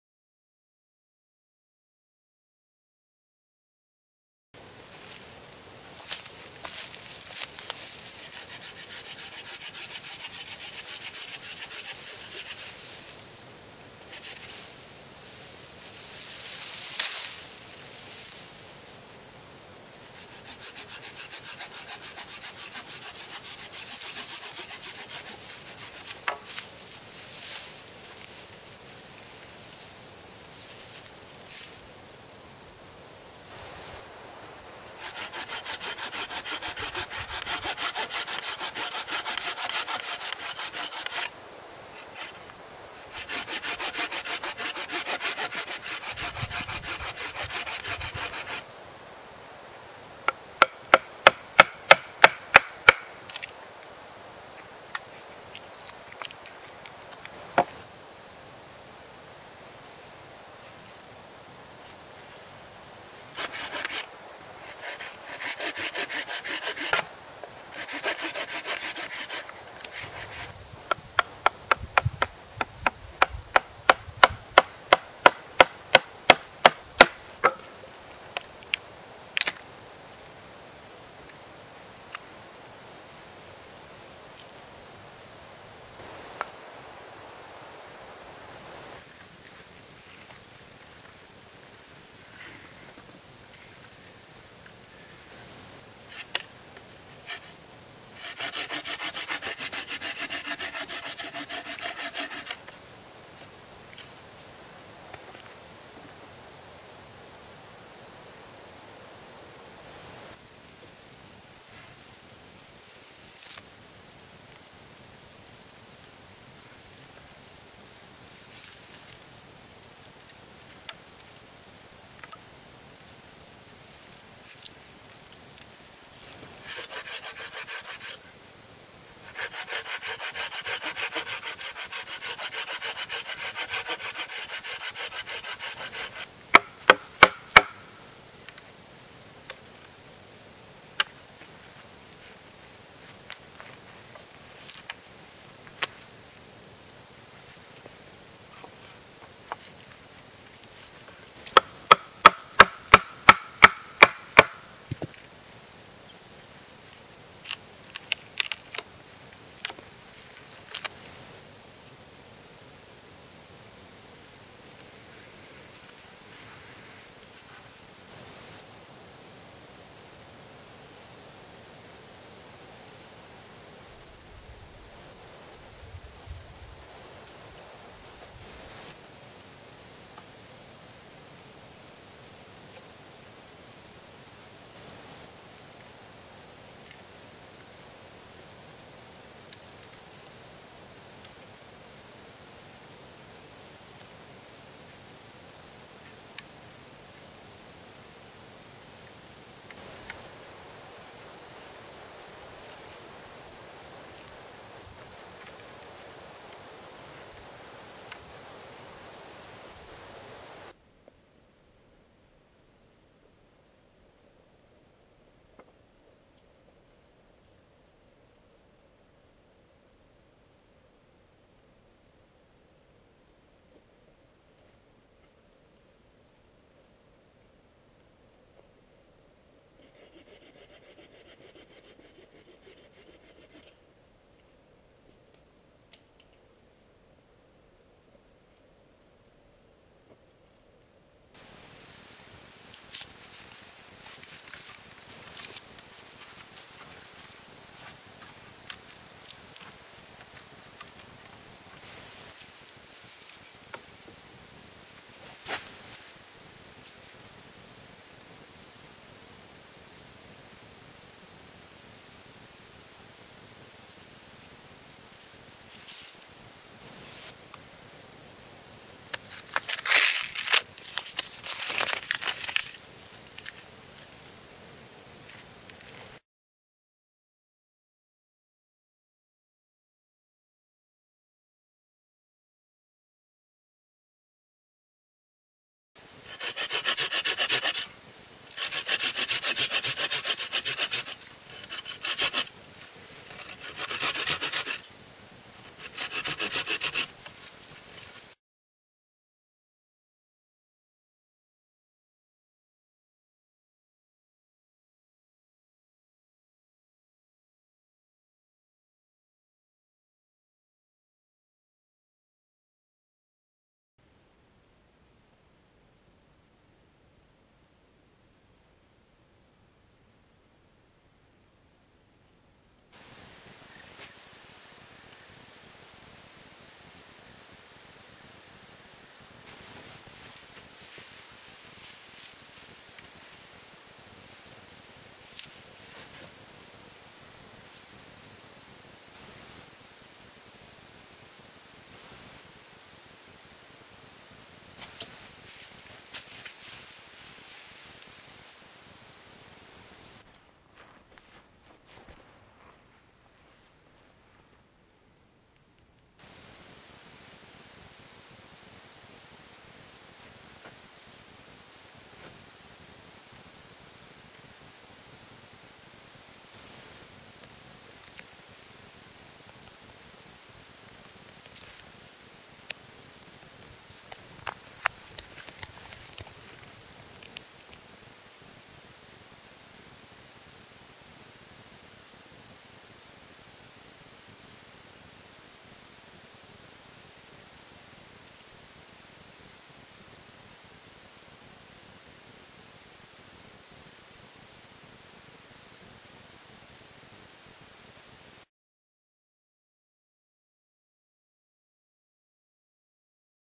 How To Build A Bushcraft Chair.
In this video I show you how to build a tripod chair in the woods by only using natural materials and rope.